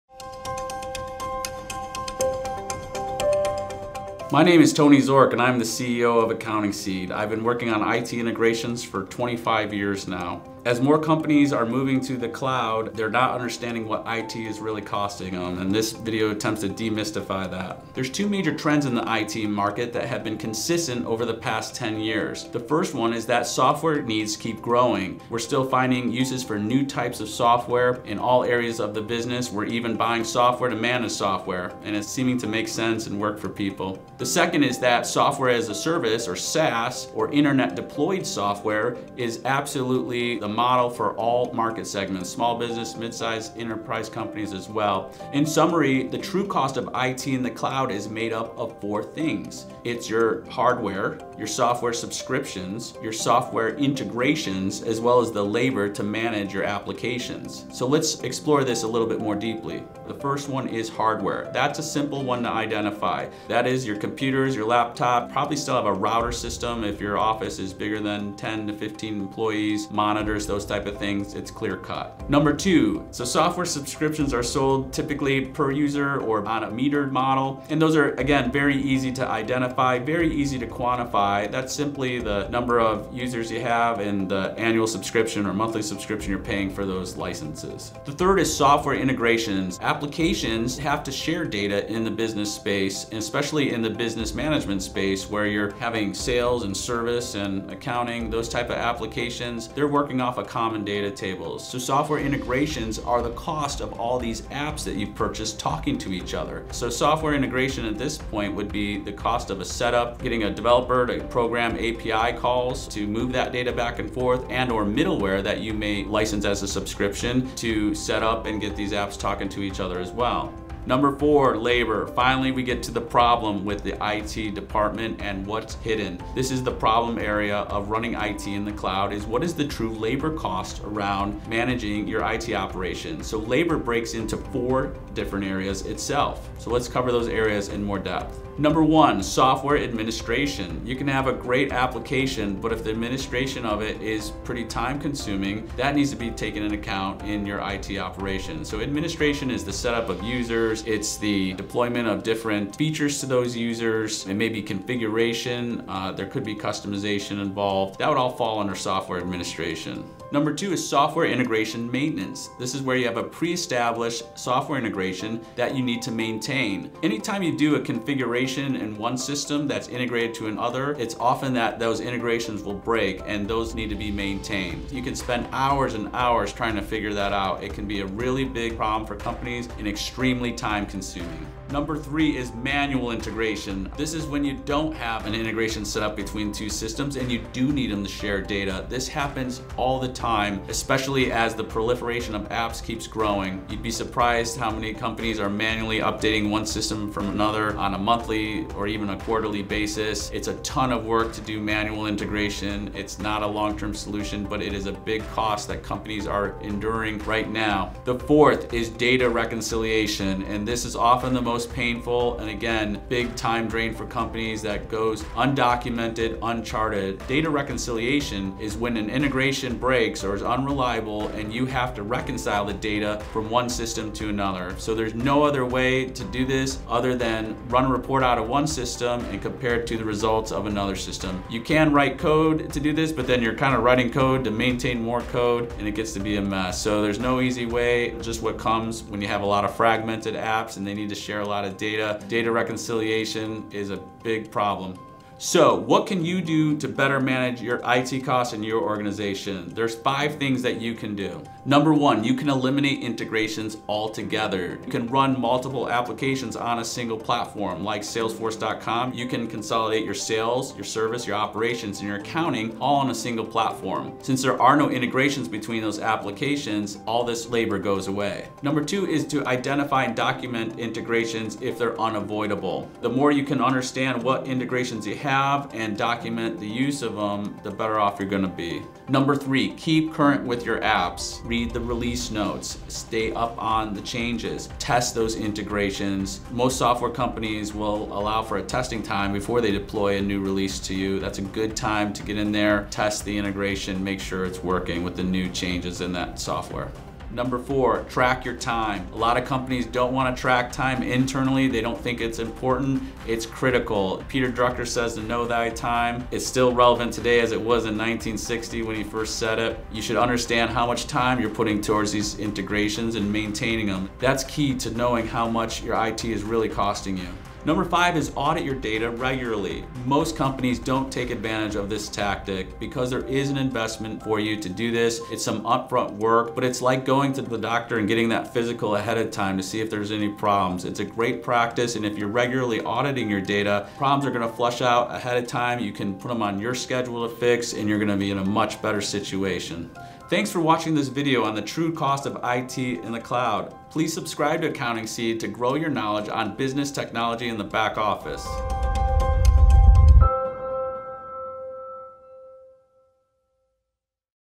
Top 5 Ways to Control Cloud IT Costs
While some expenses are shrinking in the IT world, there are still a lot of hidden costs surrounding a modern cloud IT environment. What are the essential costs? Can you save money while getting the tools and support you need? We’ll answer these questions and more as we explore the Top 5 Ways to Control Cloud IT Costs.

-For a deeper look, be sure to check out our companion blog on The True Cost of Cloud IT

AccountingSeed.com